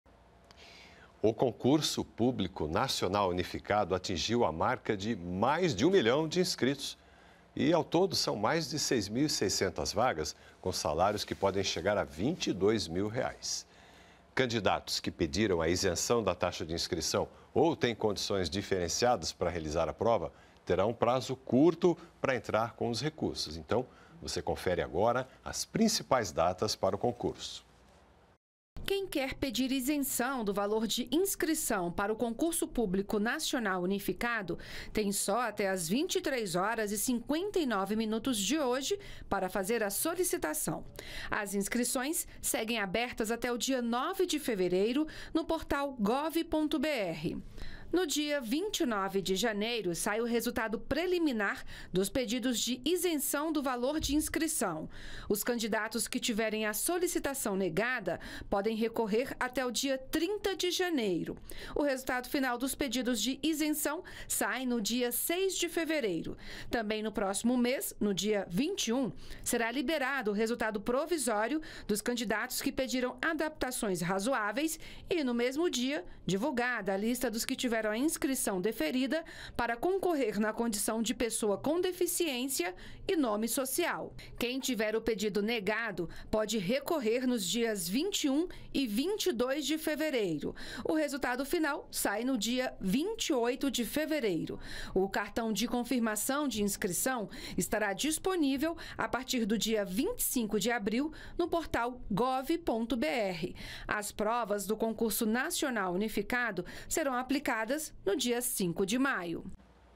CONCURSO PÚBLICO NACIONAL UNIFICADO SUPERA MARCA DE 1 MILHÃO DE INSCRITOS
O Concurso Público Nacional Unificado ultrapassou a marca de 1 milhão de inscritos. Ao todo, são mais de 6.600 vagas, com salários que podem chegar a R$ 22 mil mensais. Confira o cronograma do concurso.